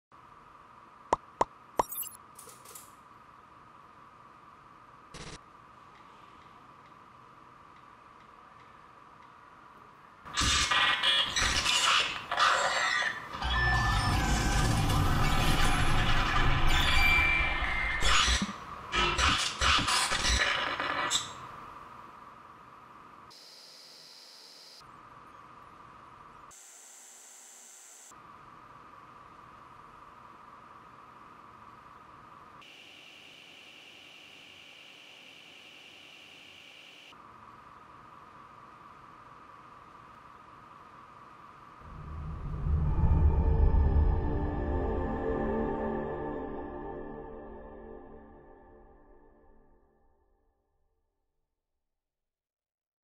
Custom Passcode Error Alarm "Security Breach Detected"
Description : Custom Passcode Error Alarm Video "Security Breach Detected"

Tweak Used: Ah! Ah! Ah! - available on BigBoss repo